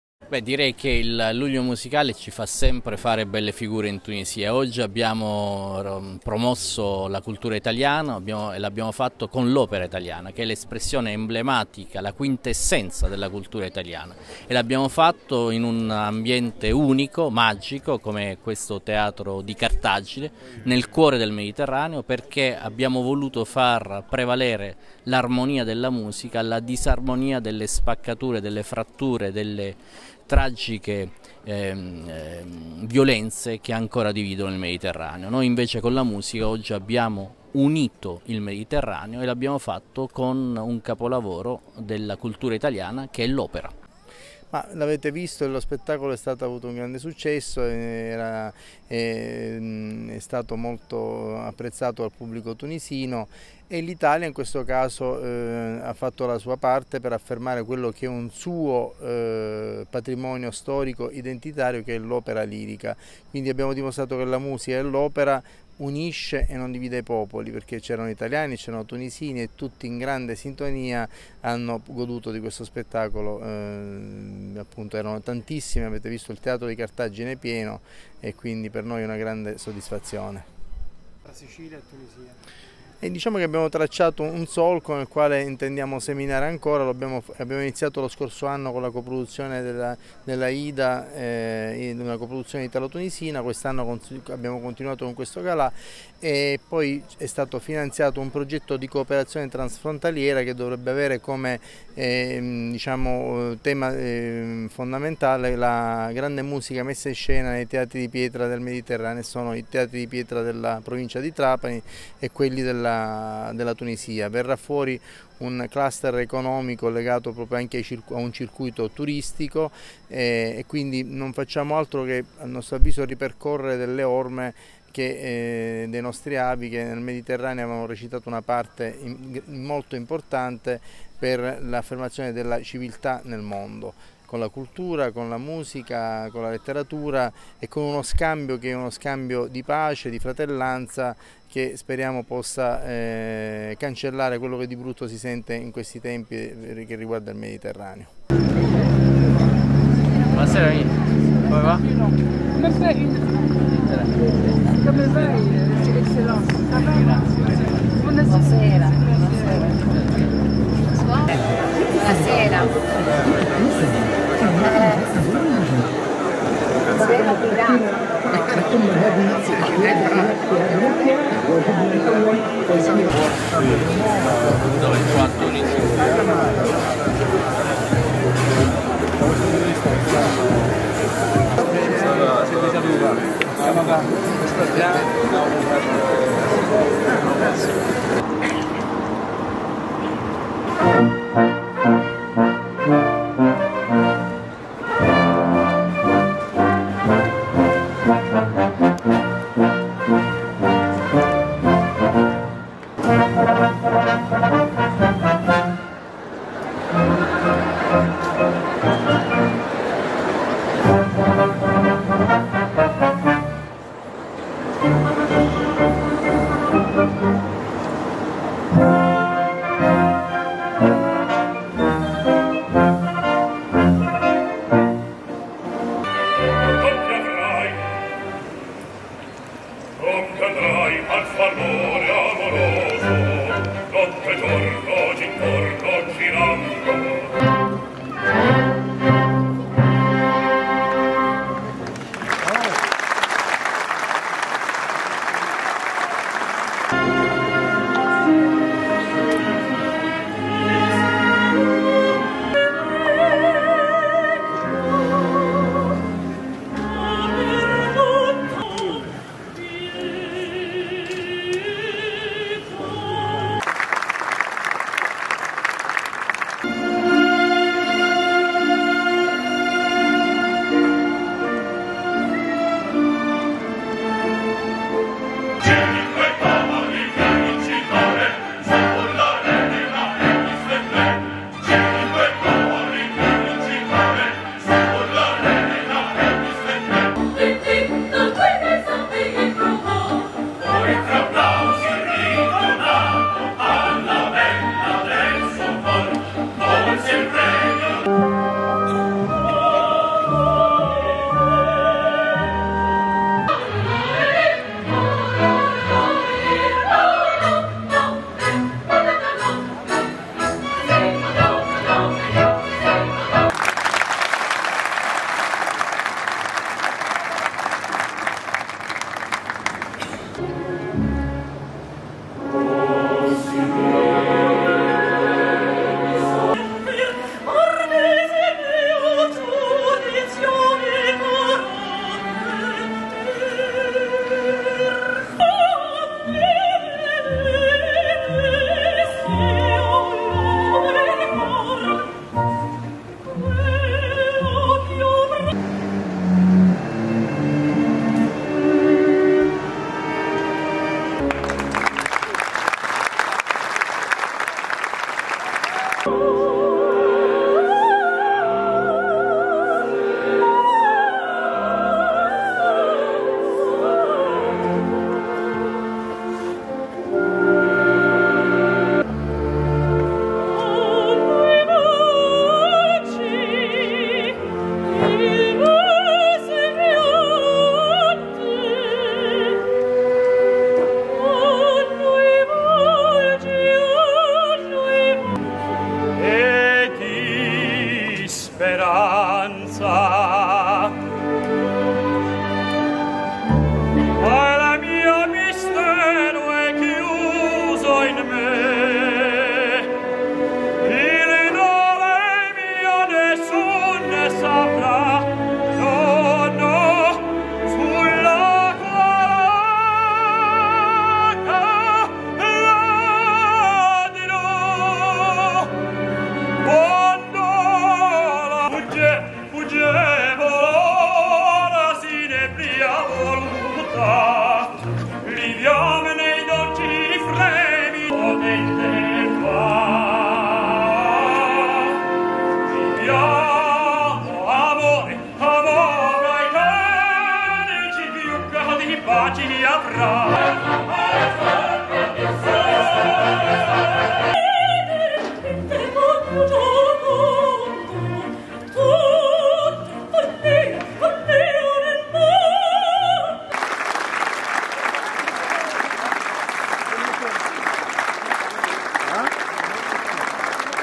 tunisia luglio
Oltre 5000 spettatori hanno riempito il teatro romano, erano presenti il consigliere delegato dell’Ente Luglio Musicale Trapanese Silvio Ermenegildo Ceccaroni e il direttore artistico Giovanni De Santis ed ancora numerosi esponenti del governo tunisino tra cui Mohamed Zine El Abidine, Ministro degli Affari Culturali, e Lorenzo Fanara, Ambasciatore d'Italia a Tunisi ed ancora altri importanti rappresentanti di istituzioni